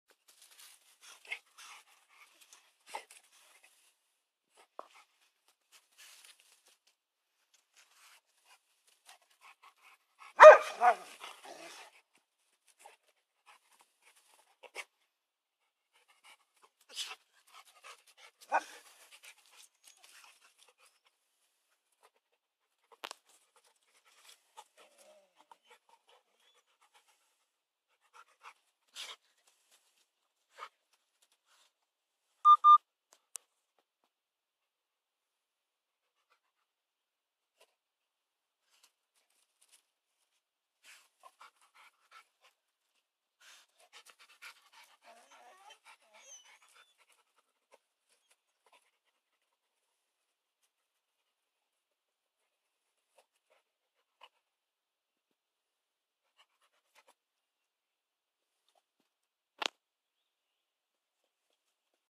A LABRADOR MEETS A HEDGEHOG
Our Labrador Shaw is the friendliest dog in the world. He would try to befriend everybody - any human and any animal. I'm sharing a video of him trying to play with a hedgehog :)